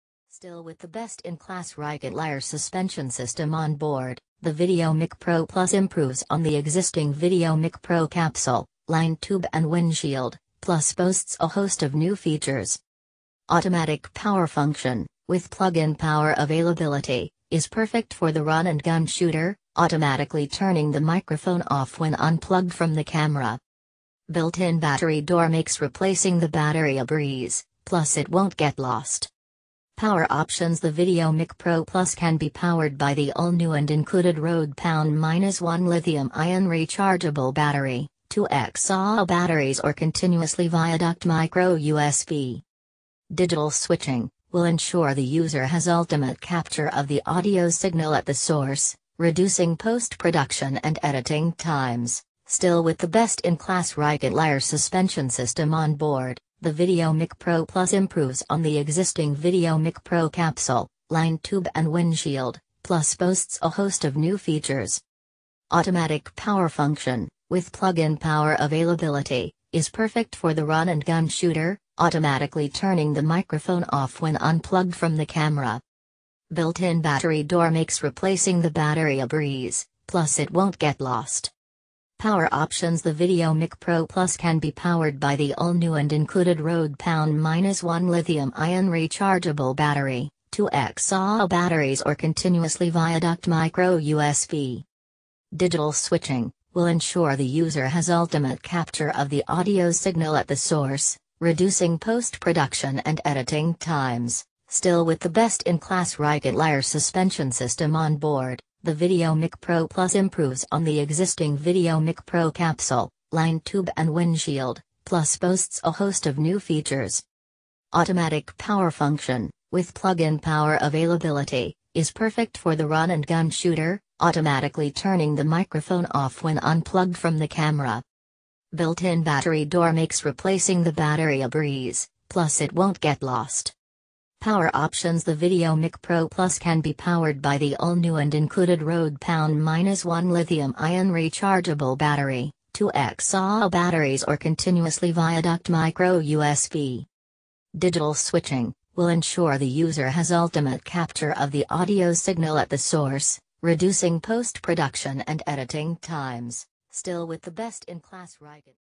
Best digital microphone | Rode VideoMic Pro+ Compact Directional On-Camera Condenser Microphone
Best digital microphone | Rode VideoMic Pro+ Compact Directional On-Camera Shotgun Condenser Microphone
Link to buy

Still with the best-in-class Rycote Lyre suspension system onboard, the VideoMic Pro+ improves on the existing VideoMic Pro capsule/line tube and windshield, plus boasts a host of new features.

Automatic power function (with plug-in power availability) is perfect for the run-and-gun shooter, automatically turning the microphone off when unplugged from the camera.

Built-in battery door makes replacing the battery a breeze - plus it won't get lost.

Power options the VideoMic Pro+ can be powered by the all-new and included Rode LB-1 lithium-ion rechargeable battery, 2 x AA batteries or continuously via micro USB.

Digital switching - will ensure the user has ultimate capture of the audio signal at the source, reducing post production and editing times.